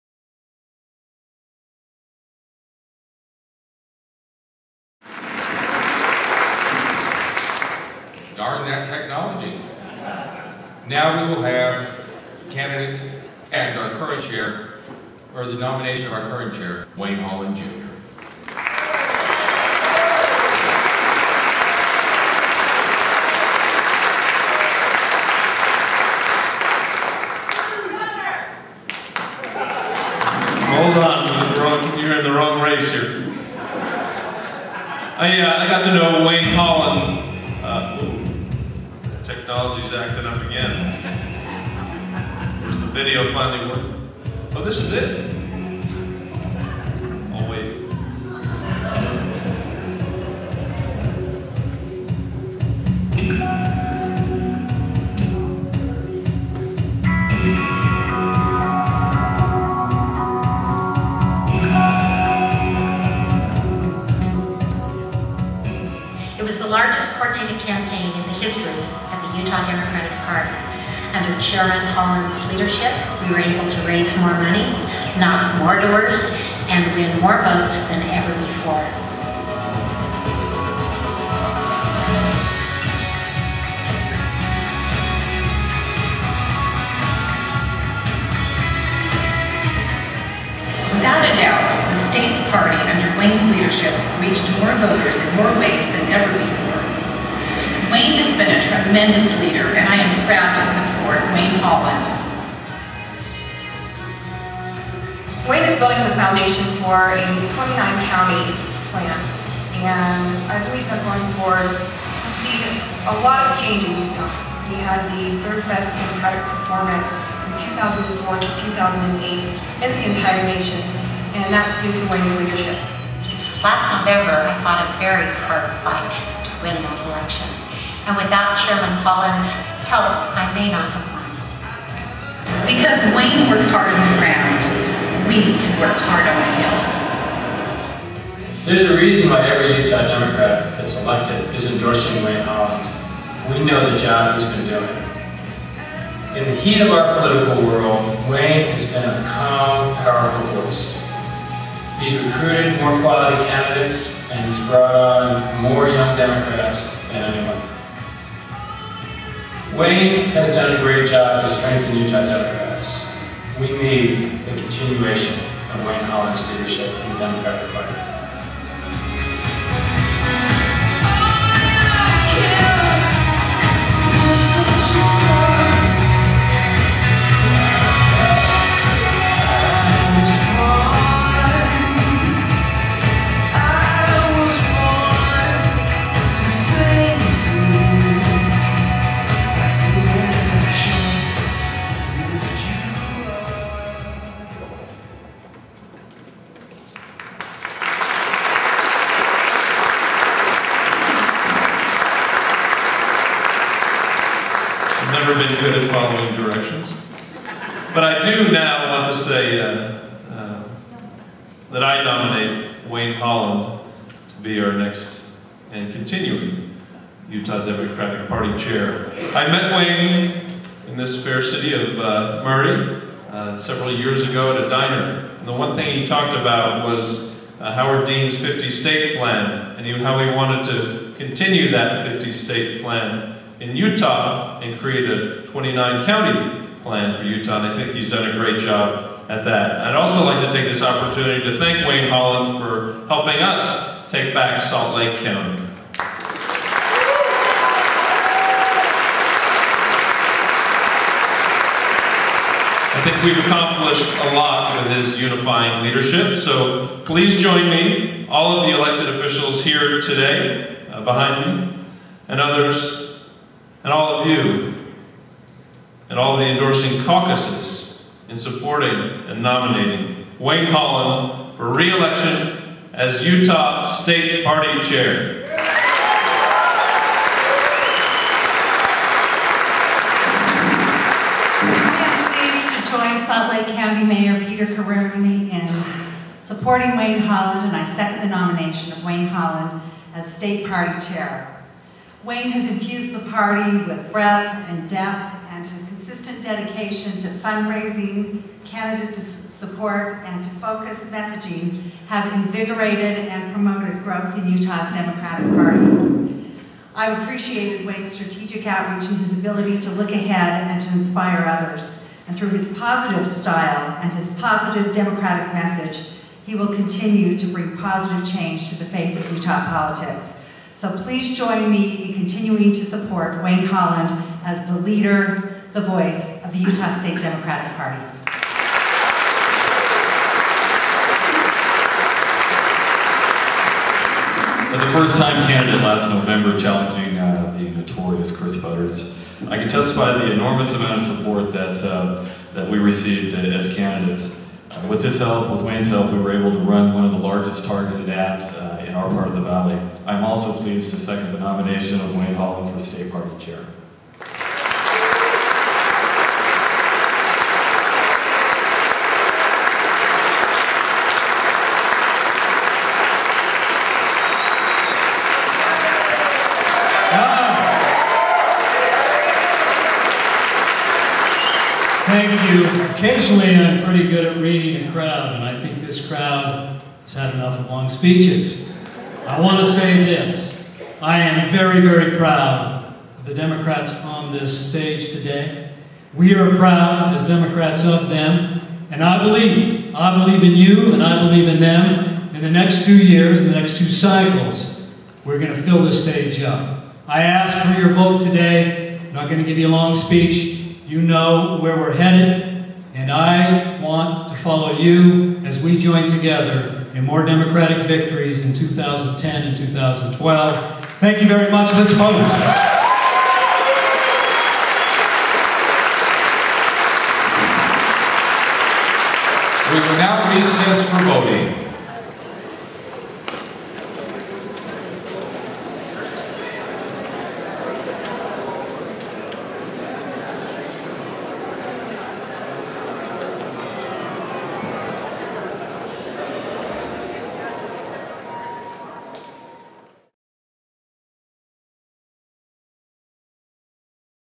2009 Utah Democratic Party Convention - Incumbent Chair Candidate Wayne Holland
Peter Corroon, Karen Hale, and John Rendell endorse incumbent Wayne Holland as candidate for State Party Chair.  Wayne Holland addresses the 2009 Utah State Democratic Party Organizing Convention, June 20 2009, Murray UT